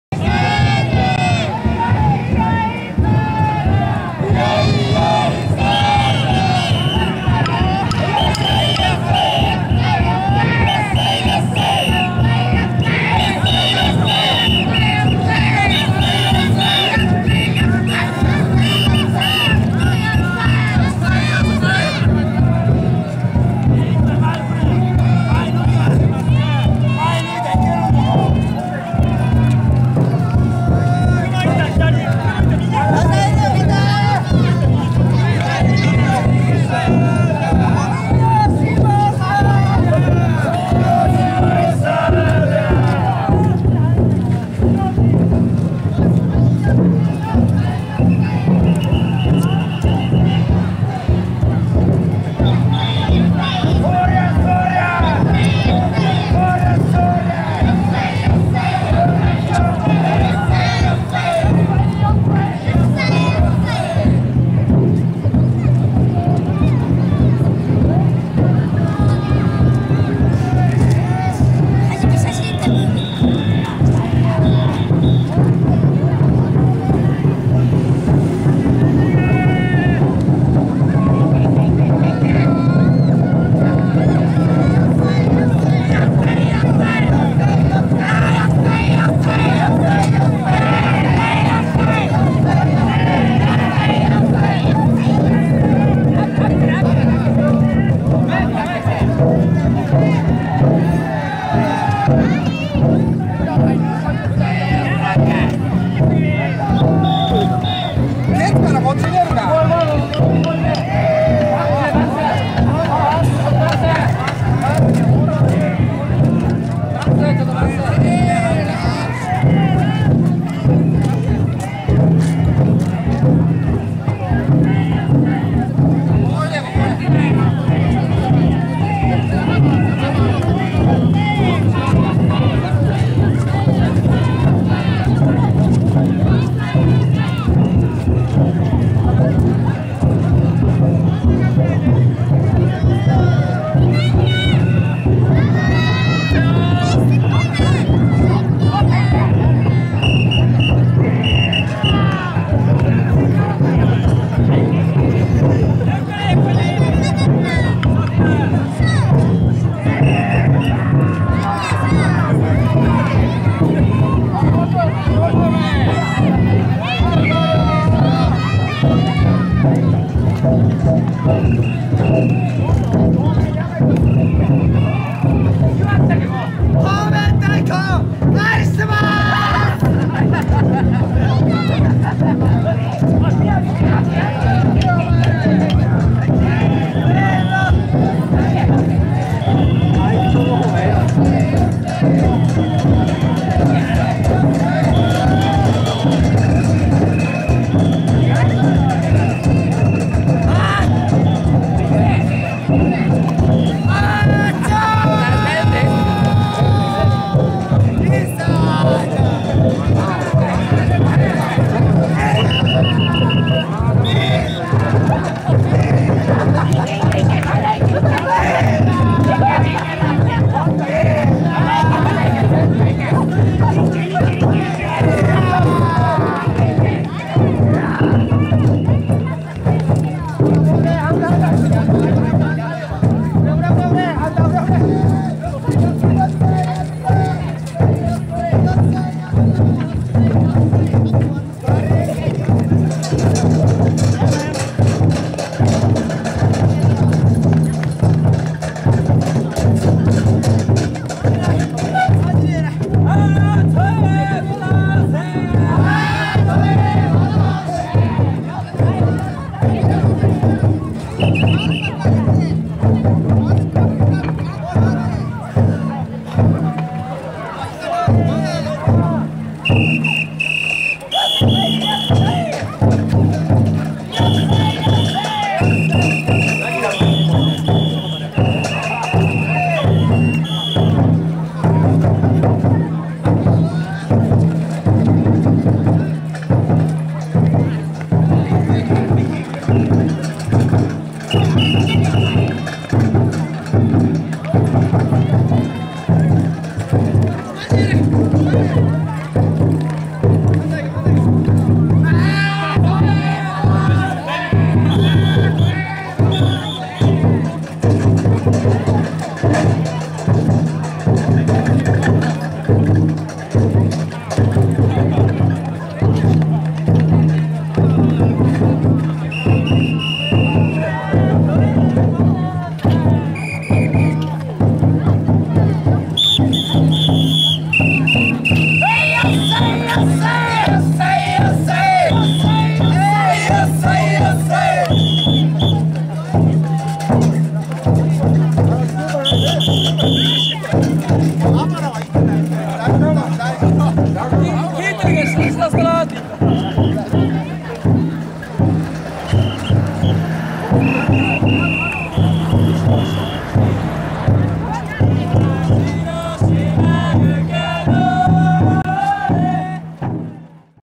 【高屋祭】午後の県道付近の様子(H26)
県道に新しく出来たファミリーマート付近での運行の様子。
各太鼓台が集まり始めました。